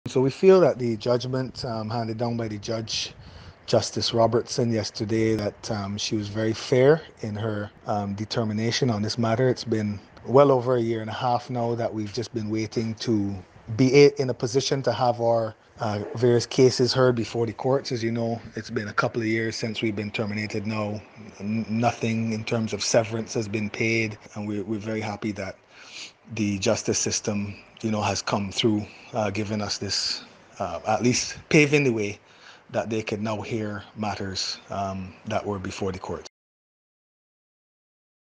Court rules in favour of LIAT pilots
The High Court of Antigua has ruled in favour of five LIAT pilots who challenged the constitutionality of a section of the Companies Amendment Act, that imposed an automatic stay of proceedings on all matters against the airline company.

This led to a 2015 High Court claim by the pilots, which was set for trial in 2020, being adjourned indefinitely.

Information received through the attorney for the pilots, Ruggles Fergusson, a senior attorney from Grenada, stated the High Court Judge, Justice Robertson, agreed with the key contentions of the pilots.

Speaking to CBC NEWS, one of the pilots in the suit Neil Cave, said he is happy with the ruling.